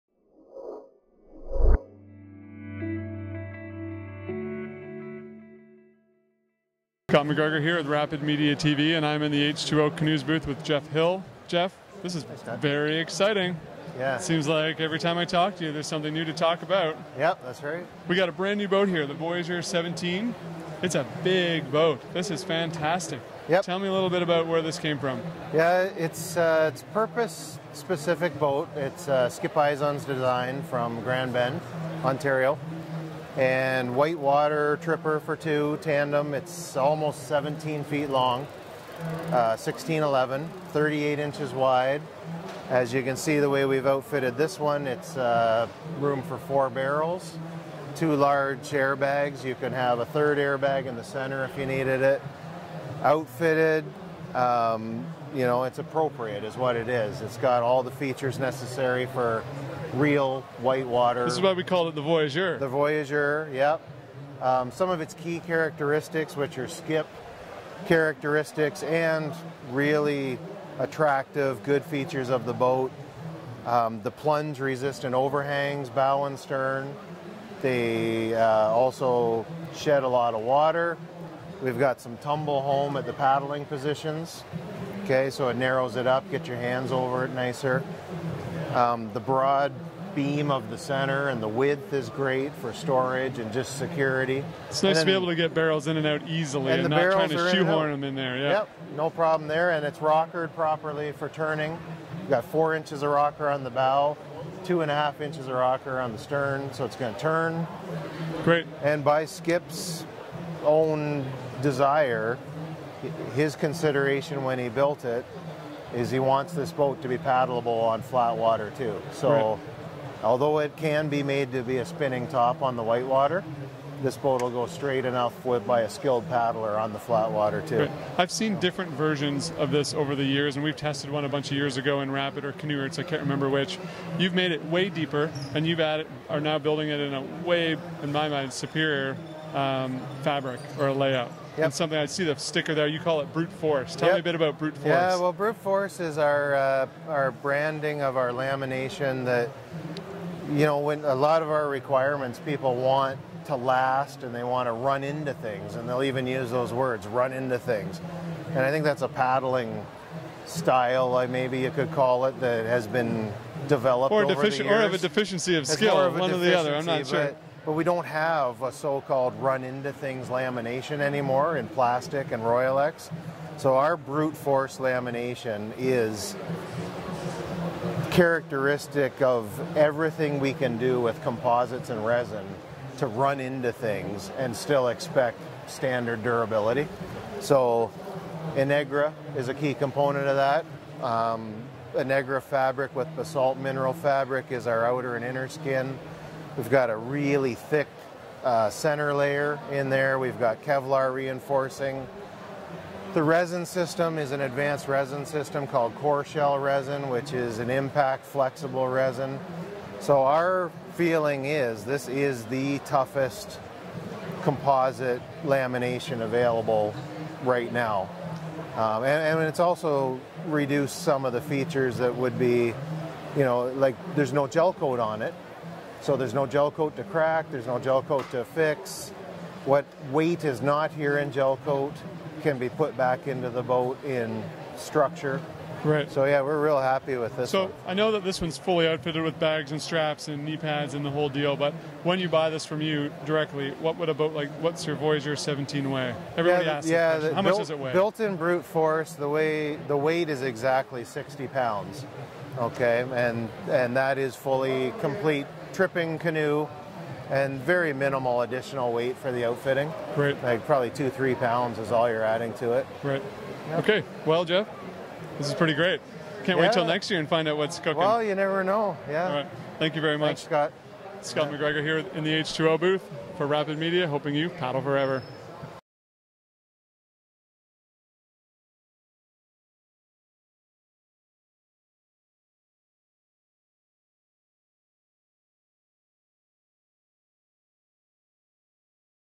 H2O's New Voyageur and Brute Force Lay-Up | Canoeroots | Rapid Media
Most exciting is H2O's new Voyageur 17, a purpose-specific boat designed to be a whitewater tripper for two. It's 16'11" and 36 inches wide. At that size it offers room for four barrels and as many as three air bags. Four inches of rocker in the bow means this canoe will be easy to maneuver. It's got all the features necessary for real whitewater adventure. Offered in the Brute Force lay-up, this so-called run-into-things lamination promises to be super durable and includes Innegra and basalt. Hill claims it's the toughest composite lamination available right now.